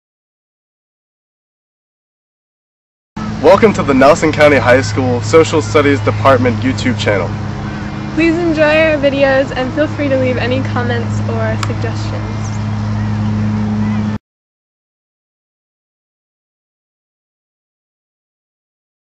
Welcome Video
Welcome to the Nelson Coutny High School Social Studies Department YouTube Site. A work in progress, this site features student documentaries, short skits, dbq's, and other projects aligned with our Social Studies curriculum.